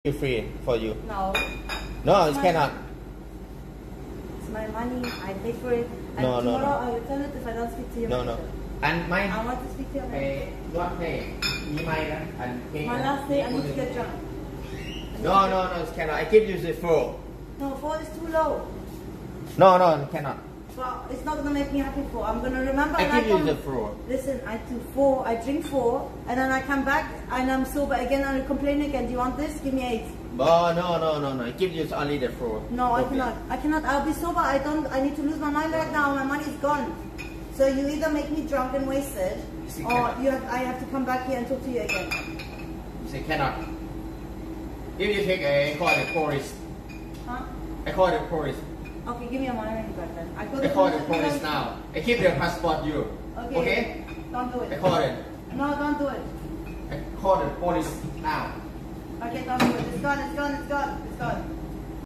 Broken Jacuzzi! I Want a refund or all the beer!
Shako Mako Kick IRL in Laos almost gets arrested trying to get a refund on a room with a broken jacuzzi.